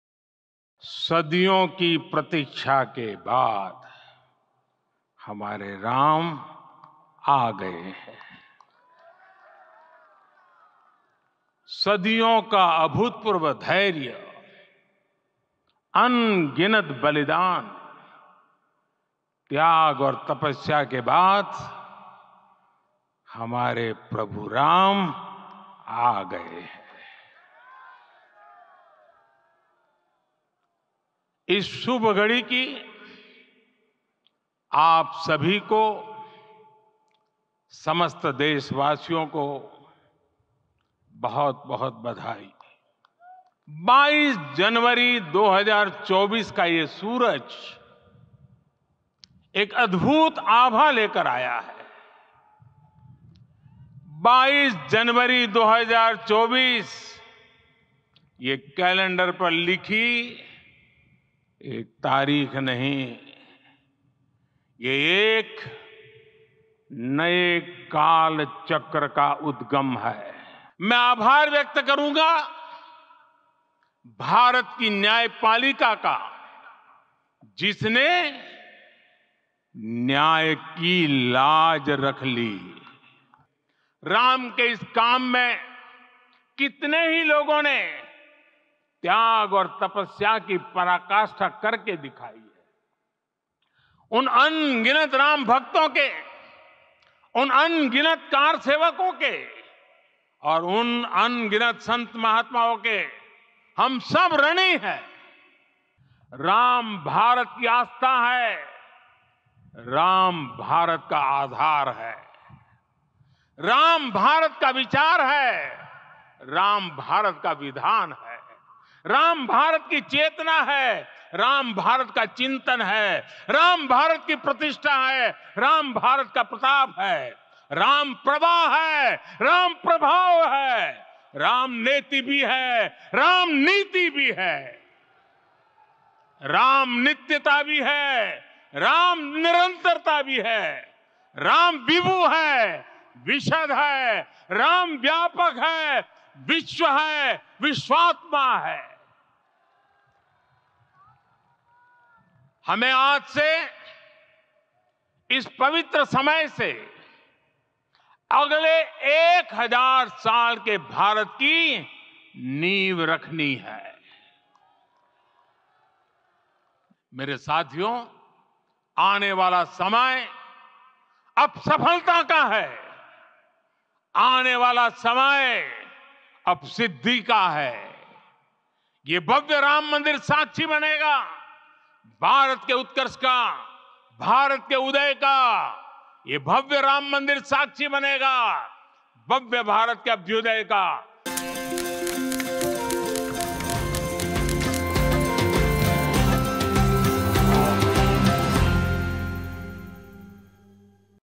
Ram Mandir Pran Pratishtha: PM मोदी का मंदिर परिसर से देश को संदेश| NDTV Profit Hindi
Ayodhya के भव्य Ram Mandir में रामलला की Pran Pratishtha संपन्न हुई. प्रधानमंत्री Narendra Modi ने 84 सेकेंड के अभिजीत मुहूर्त में पूरे विधि-विधान के साथ प्राण-प्रतिष्‍ठा का अनुष्‍ठान पूरा किया.  प्राण-प्रतिष्‍ठा के बाद PM मोदी ने मंदिर परिसर से देश को संबोधित किया. बोले हमारे रामलला अब टेंट में नहीं रहेंगे, वे दिव्‍य मंदिर में रहेंगे. देखें प्रधानमंत्री के speech की Key Highlights इस वीडियो में.